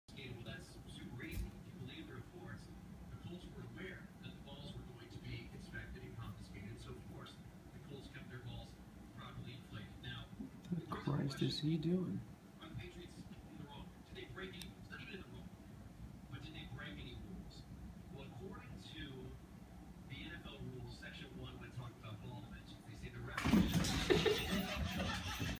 Rhodesian Ridgeback the Lion Hunter
11 week old Rhodesian Ridgeback Dexter stalking Boston Terrier.  Dexter genes must be confusing the terrier with a Lion.